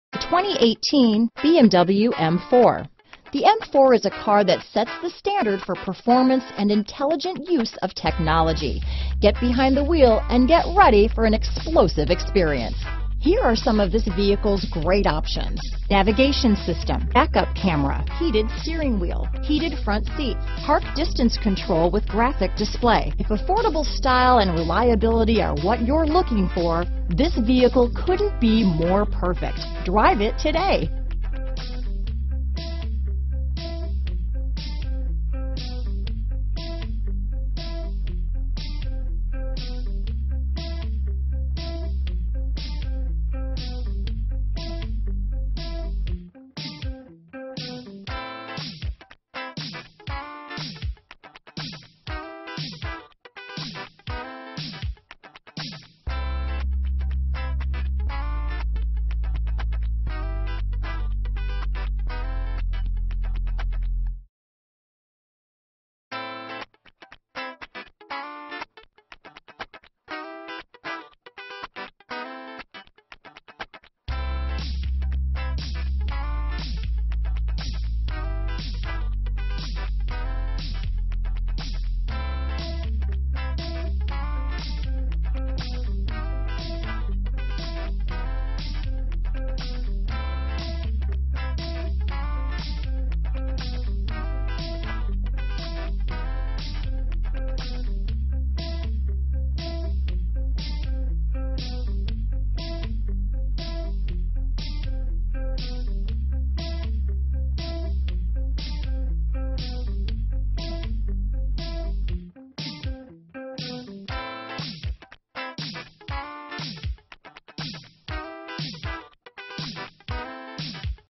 2018 BMW M4 San Francisco, San Jose, Oakland, Marin, bay area, CA 18044
2018 BMW M4

WHEELS: 20  X 9.0  FRONT & 20  X 10.0  REAR BLACK  -inc: M forged  star-spoke  style 666M,TRANSMISSION: 7-SPEED M-DOUBLE CLUTCH W/DRIVELOGIC,PARK DISTANCE CONTROL,EXECUTIVE PACKAGE  -inc: Speed Limit Info  Automatic High Beams  Head-Up Display  Side & Top View Cameras  Parking Assistant  Icon Adaptive Full LED Headlights,COMPETITION PACKAGE  -inc: Tires: P265/30R20 Front & P285/30R20 Rear Mixed performance  Engine: 3.0L 6-Cylinder TwinPower Turbo  automatic start/stop function w/444 HP  M Competition Package  new settings for suspension  DSC and Active M Differential  Wheels: 20  x 9  Front & 20  x 10  Rear M forged  star-spoke  style 666M  Competition Package (7MA)  BMW Individual High-Gloss Shadow Line  extended controls,ACTIVE DRIVING ASSISTANT,HEATED STEERING WHEEL,POWER REAR SUNSHADE,APPLE CARPLAY COMPATIBILITY,ACTIVE BLIND SPOT DETECTION,Body-Colored Power Heated Auto Dimming Side Mirrors w/Manual Folding and Turn Signal Indicator,Speed Sensitive Rain Detecting Variable Intermittent Wipers w/Heated Jets,Heated Front Seats,Sport Leather Steering Wheel,Leather/Metal-Look Gear Shift Knob,Navigation System,4-Wheel Disc Brakes w/4-Wheel ABS  Front And Rear Vented Discs  Brake Assist and Hill Hold Control,Tire Specific Low Tire Pressure Warning,Back-Up Camera,600w Premium Amplifier